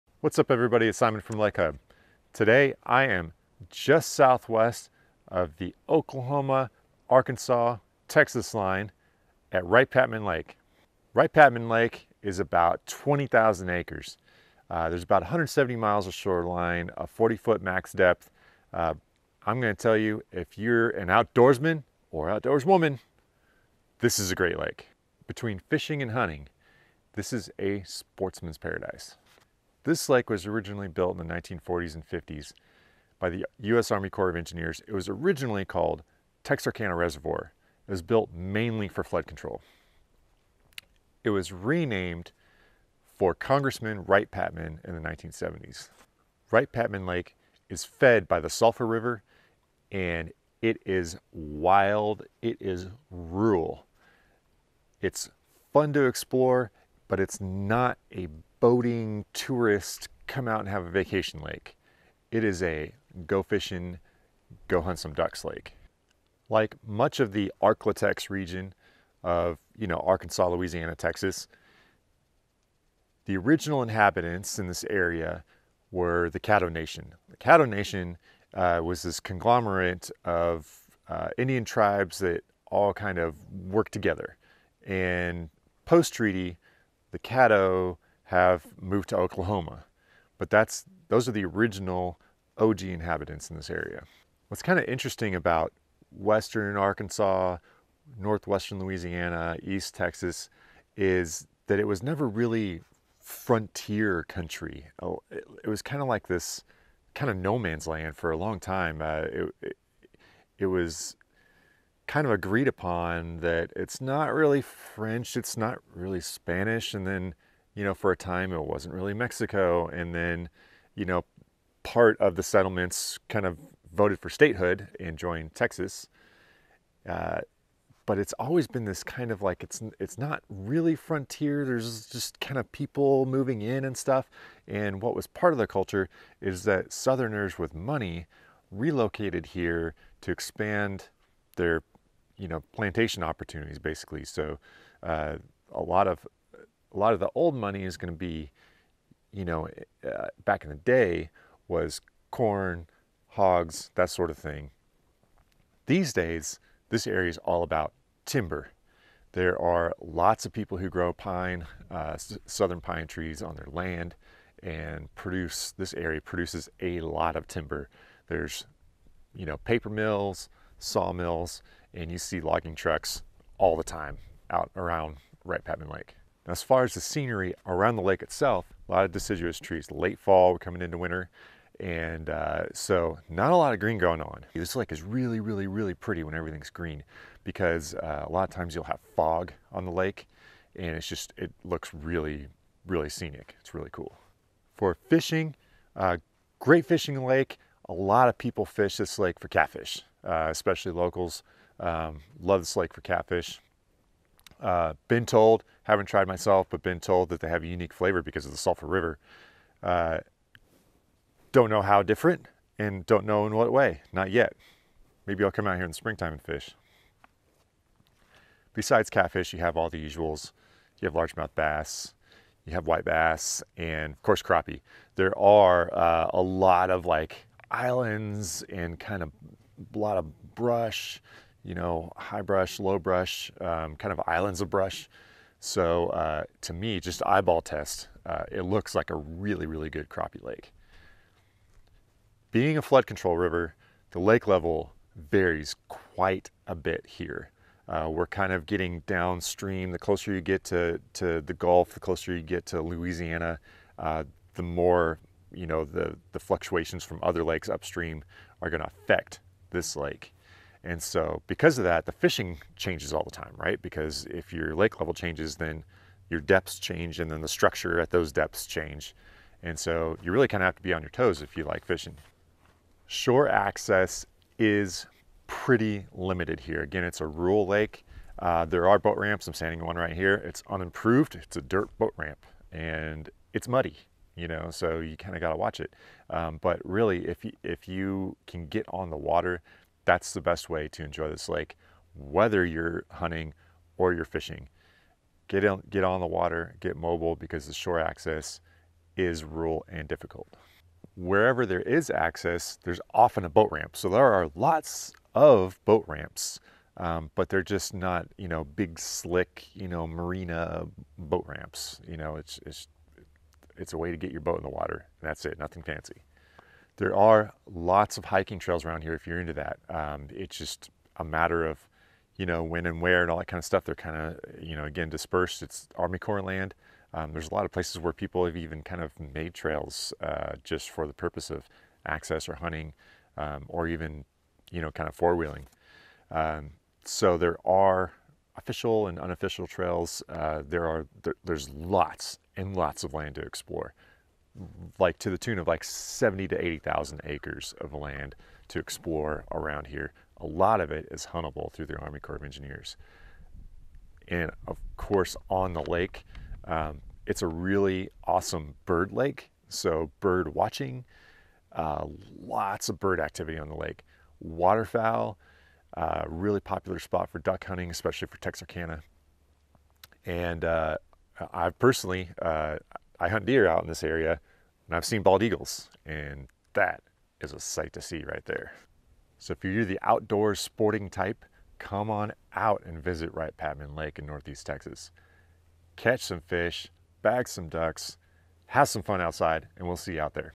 Spotlight on Wright Patman Lake: Visitor's guide, history, boating, hunting, fishing and scenery
This video is about Wright Patman Lake in North East Texas. This is a sportsman's paradise, with over 20,000 acres of hunting and fishing waterways, and over 70,000 acres of wooded shore to explore.

In this video:
0:00 - About Wright Patman Lake
0:29 - History of Wright Patman Lake
2:56 - Scenery Around Wright Patman Lake
3:14 - Fishing Wright Patman Lake
4:32 - Access and Boat Ramps
5:15 - Hiking Wright Patman Lake